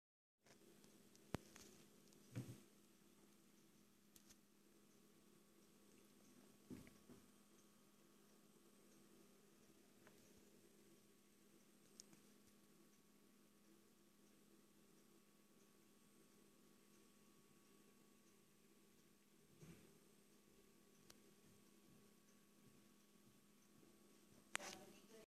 Alevines de Platy mickey azul
El mundo de los peces ecu. Alevines de platy de aproximadamente 3 meses de edad.